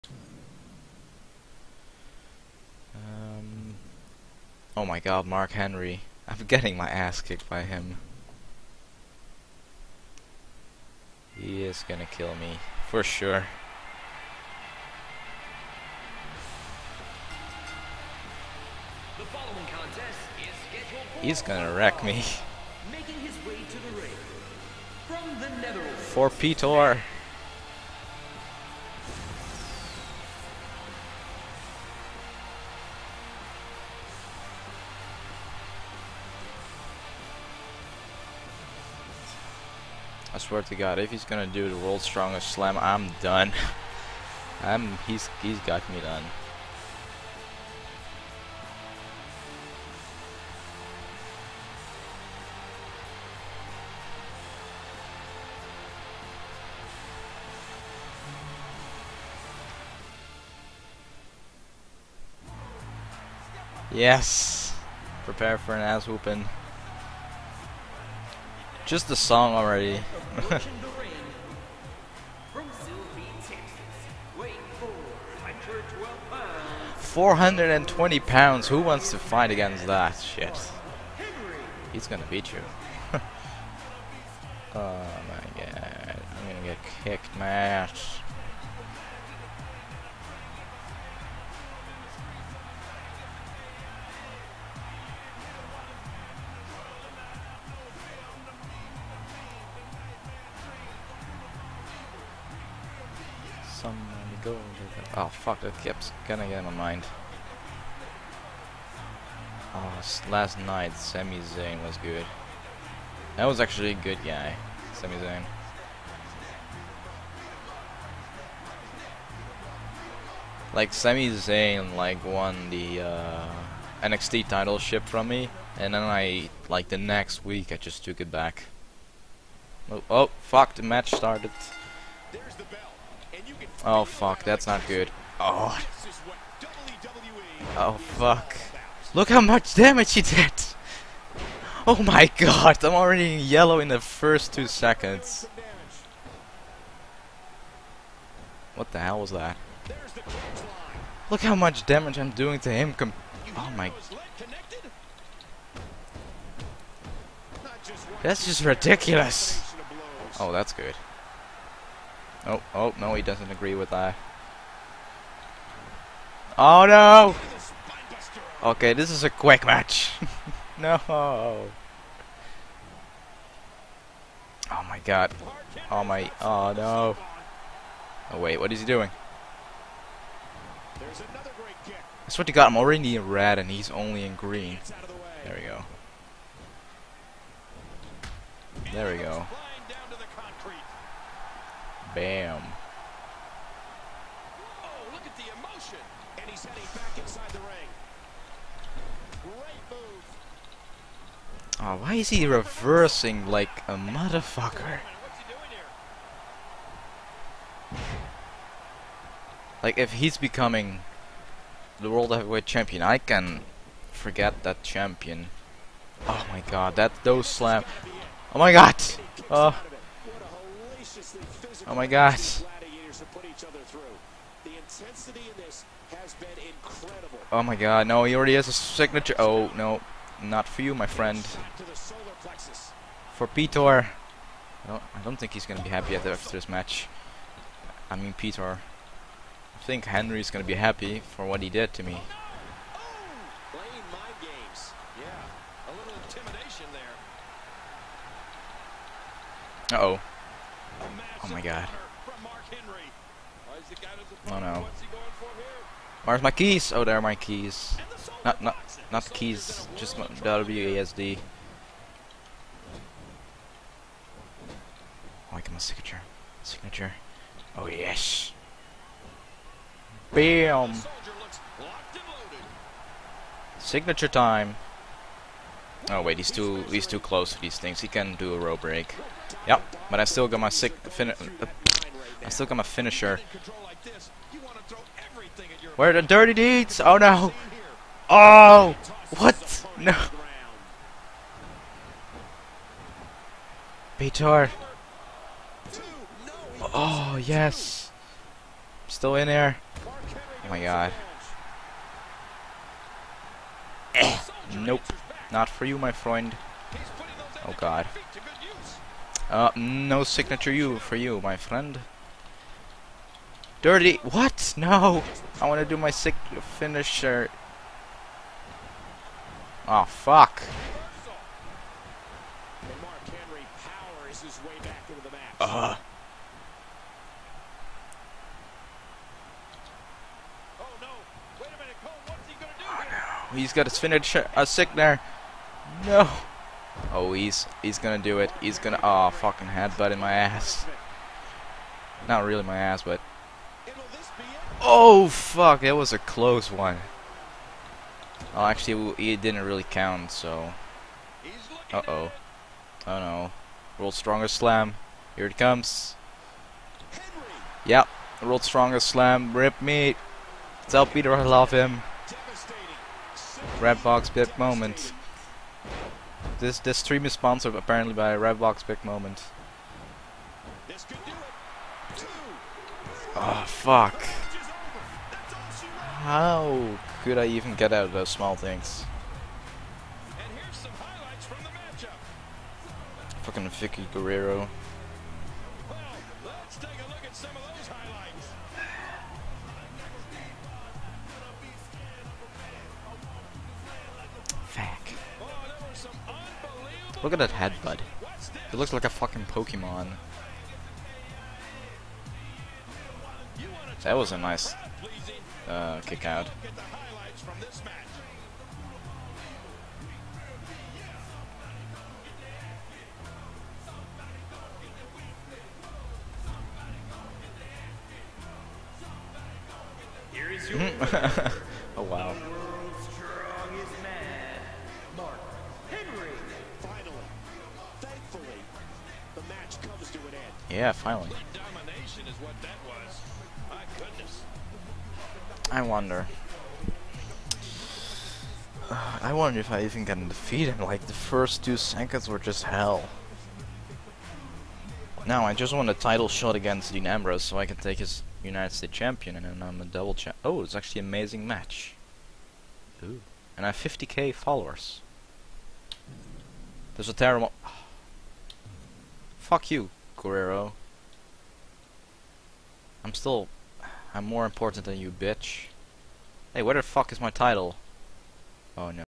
I got my ass kicked.
Note to self never lay on the floor with Mark Henry around!